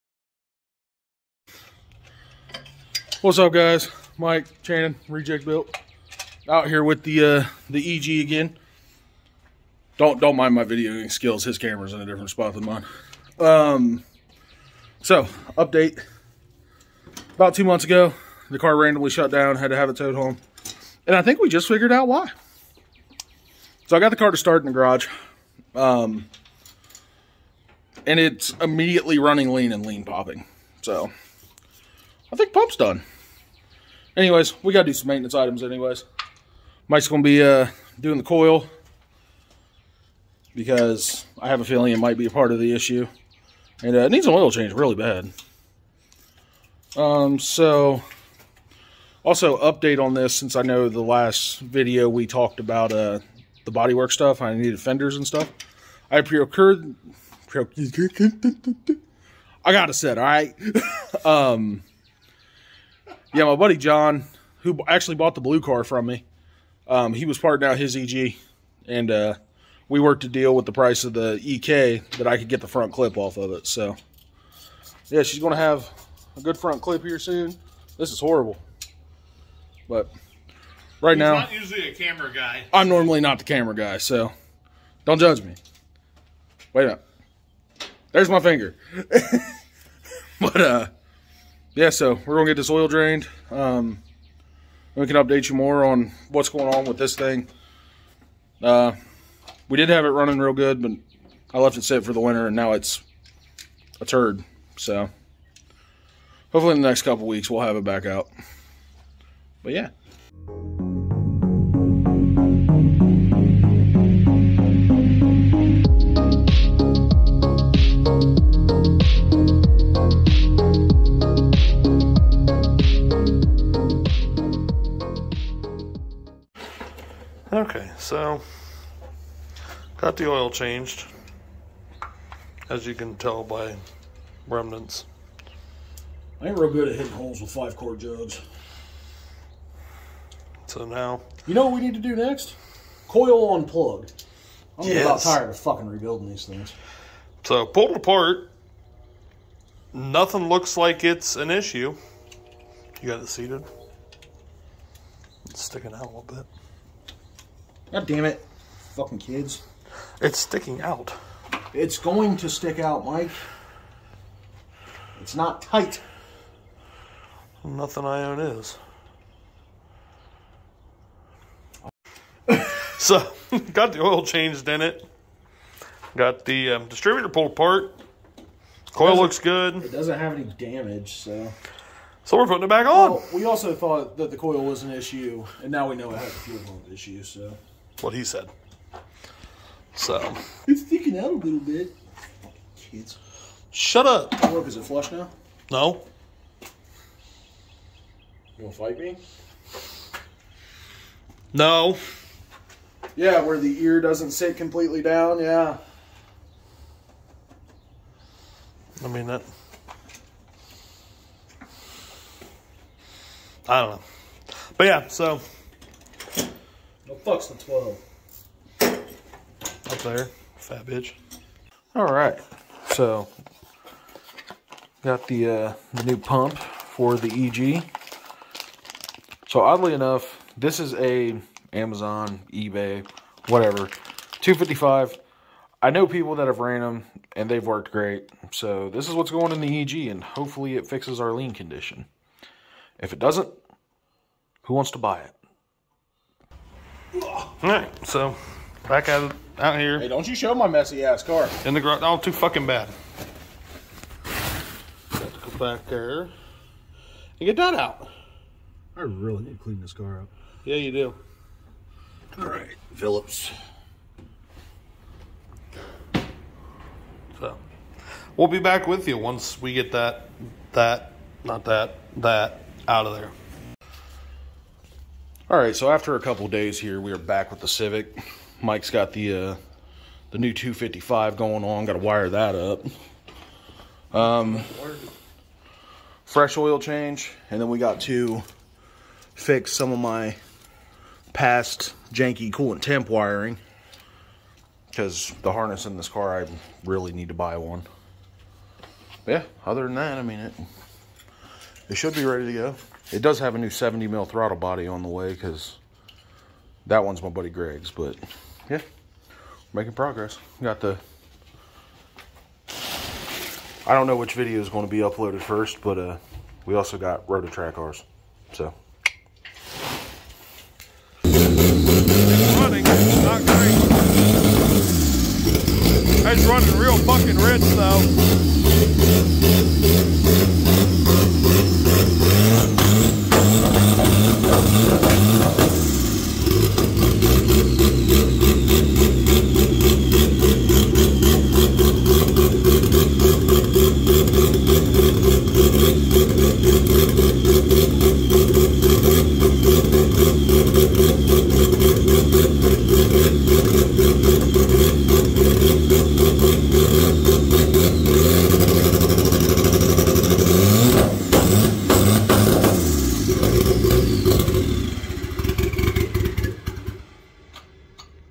update on the turbo civic
doing some work on channons turbo civic